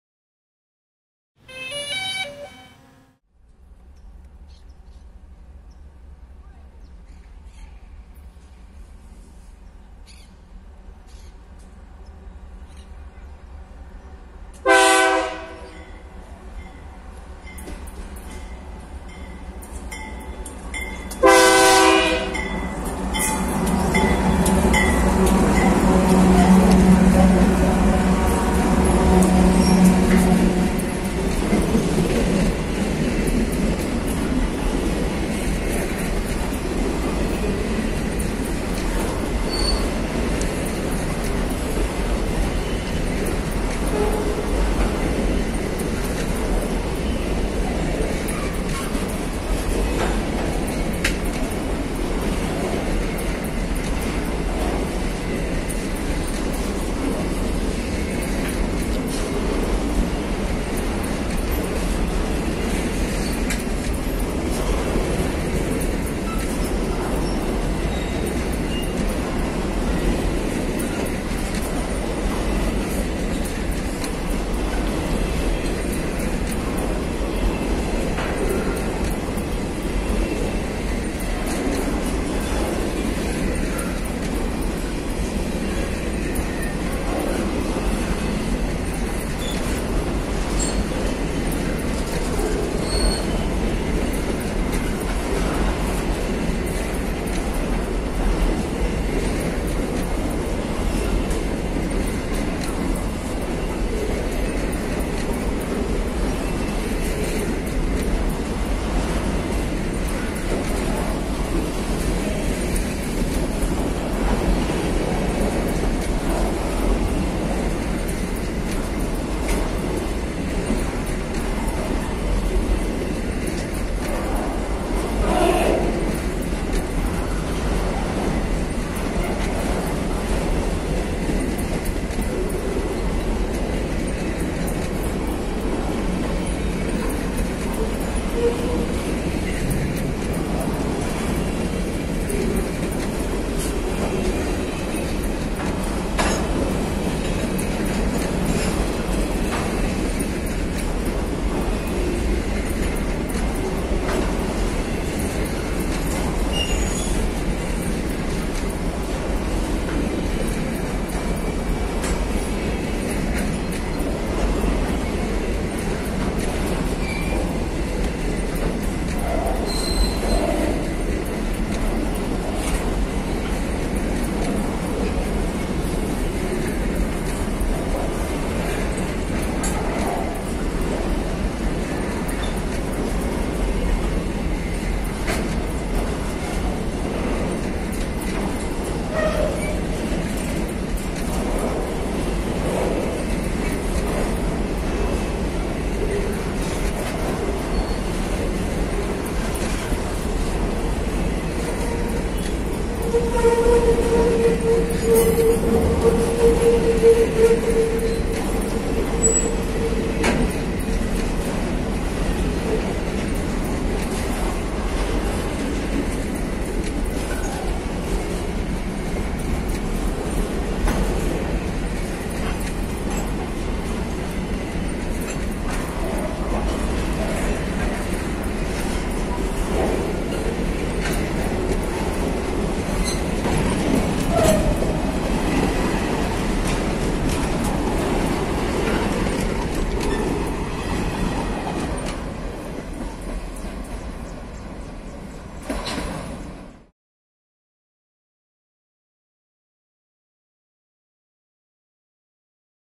Trem Vazio Passando por Votuporanga - 4K
Mais um trem vazio passando estação na cidade de Votuporanga - SP